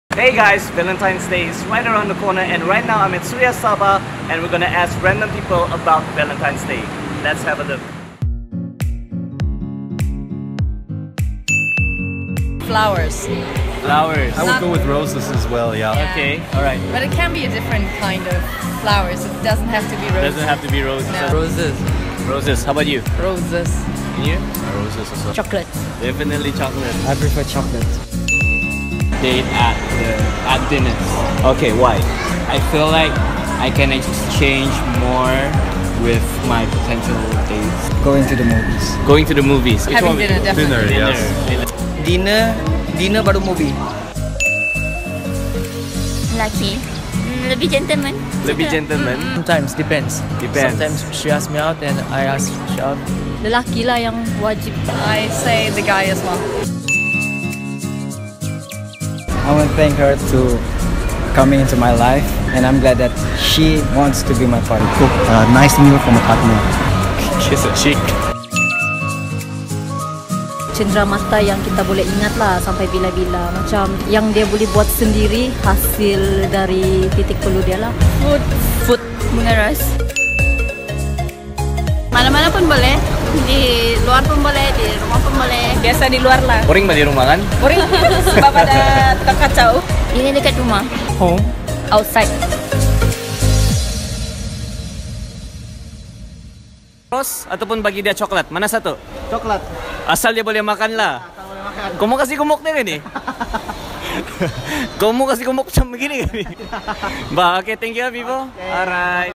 Valentine's Day | Random Interviews
Let's ask some questions to random people for this Valentine's Day 2018 at Suria Sabah Shopping Mall!

Video shot by @renjixdx
Video edited by Jelly KK